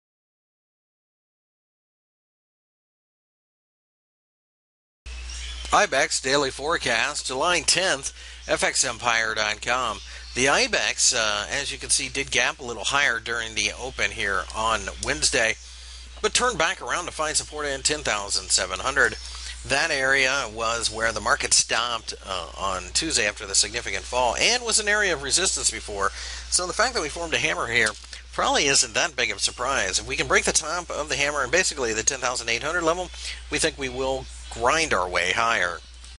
IBEX 35 Technical Analysis for July 10, 2014 by FXEmpire.com
- July 10, 2014 index daily technical analysis for the IBEX. Find more information about Forex and Commodities News Technical Analysis Fundamental Analysis and Brokers on FX Empire